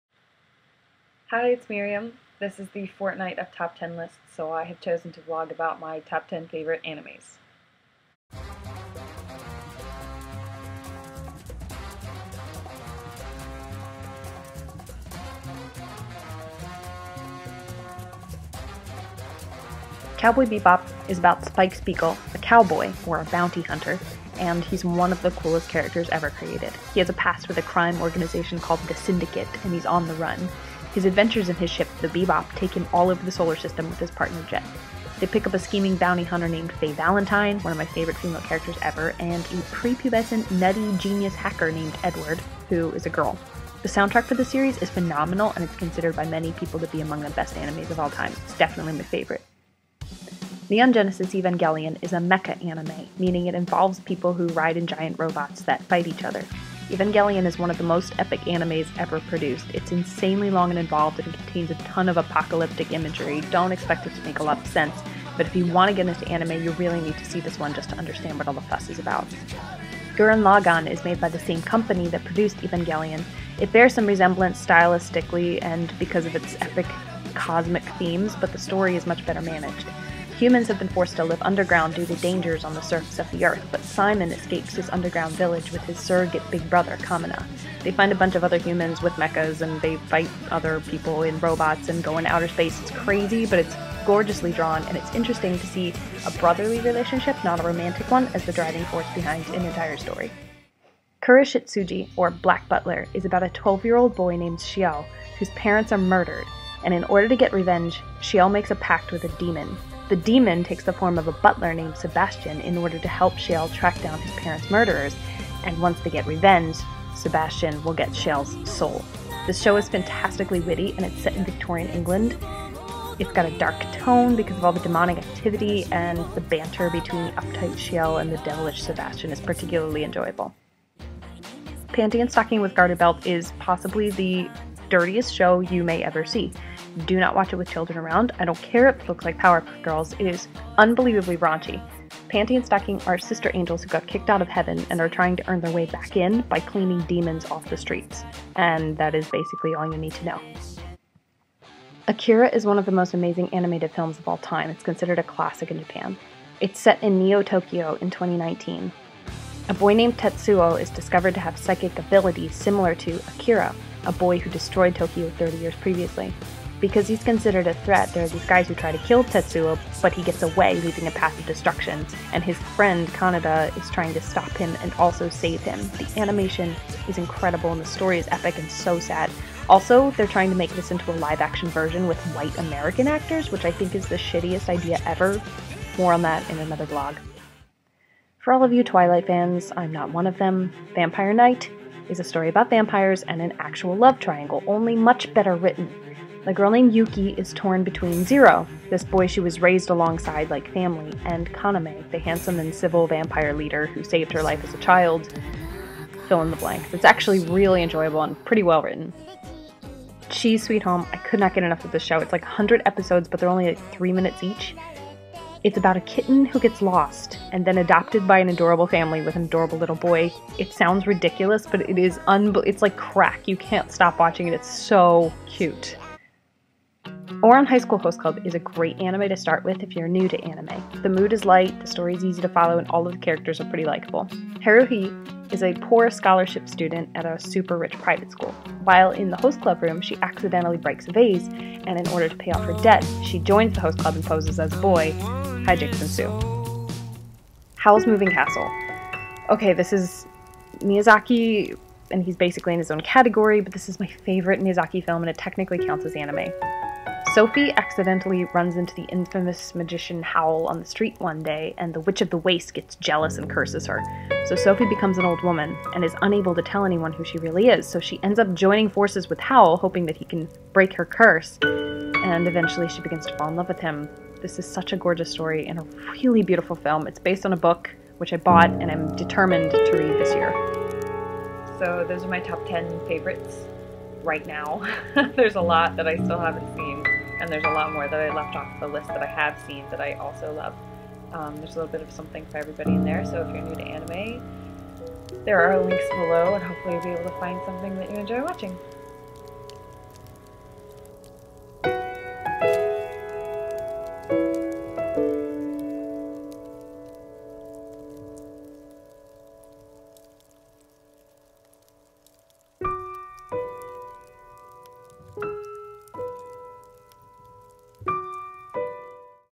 Top Ten - Animes by Miriam
Miriam lists her top ten favorite animes!

There are also animes streaming on Hulu and Netflix!